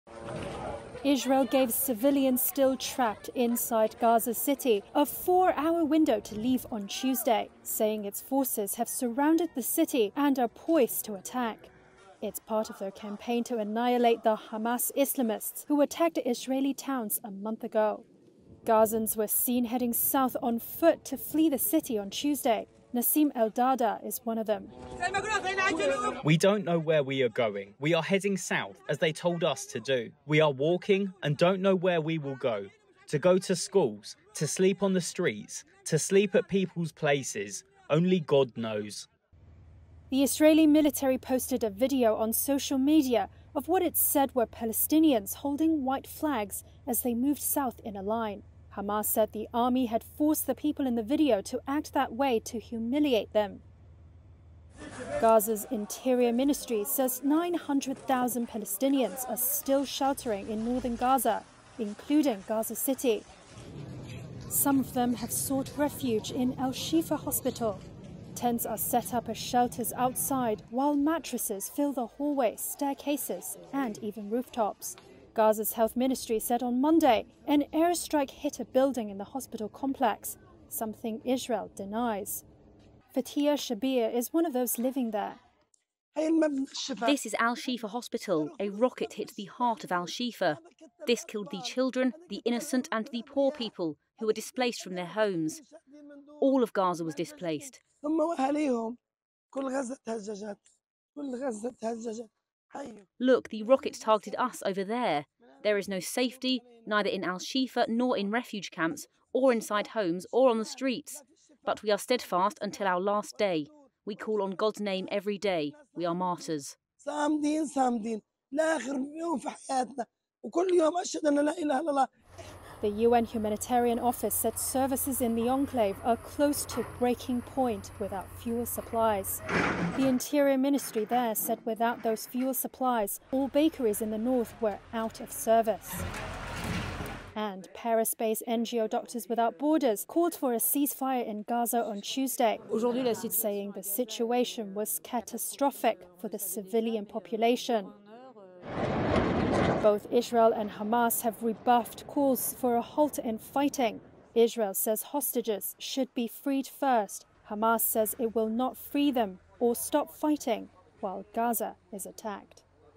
ガザ市から大勢の市民が避難、イスラエル側が4時間の退去猶予
イスラエルは７日、ガザ市内にとどまる市民に対し退去する時間として４時間の猶予を与えた。同軍はガザ市を包囲し、攻撃態勢を整えたとしている。ガザ内務省によると、ガザ市を含むガザ北部では、いまだ９０万人のパレスチナ人が避難生活を送っているという。